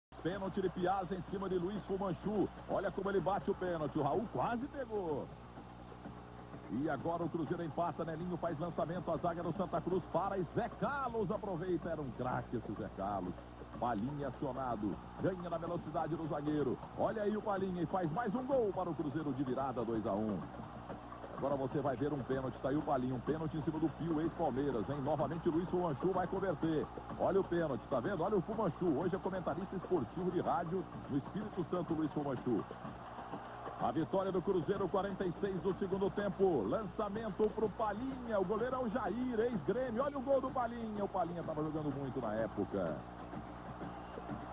Brasileirão de 1975 - Santa Cruz 2 x 3 Cruzeiro
Santa Cruz 2 x 3 Cruzeiro (07/12/1975)
Local: Arruda. Árbitro: Romualdo Arppi Filho (SP). Gols: Fumanchu aos 32 e Zé Carlos aos 43 do 1º; Palhinha aos 2, Fumanchu (pênalti) aos 28 e Palhinha aos 45 minutos do 2º tempo. Cartões Amarelos: Pedrinho e Carlos Alberto Rodrigues, Vanderlei e Palhinha. Público: 38.118 torcedores
Santa Cruz: Jair; Carlos Alberto Barbosa, Lula, Levir Culpi e Pedrinho; Givanildo, Carlos Alberto e Alfredo (Volnei); Fumanchu, Ramón e Pio. Técnico: Paulo Emílio
Cruzeiro: Raul; Nelinho, Morais, Darci Menezes e Vanderlei; Piazza, Zé Carlos e Eduardo Amorim (Isidoro); Roberto Batata, Palhinha e Joãozinho. Técnico: Zezé Moreira